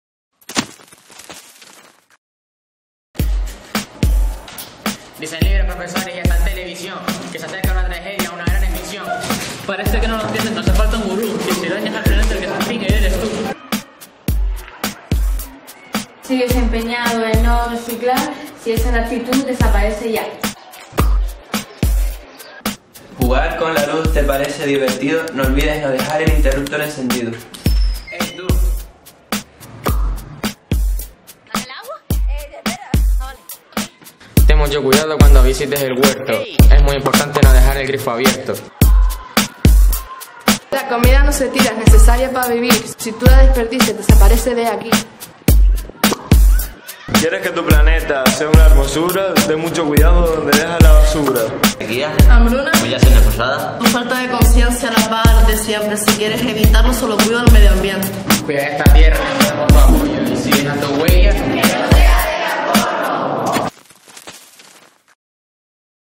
Quien se extingue eres tú.
Si no cuidas el planeta, quien se extingue eres tú.

IES José Arencibia Gil 1º ESO B
Festival de Clipmetrajes de Manos Unidas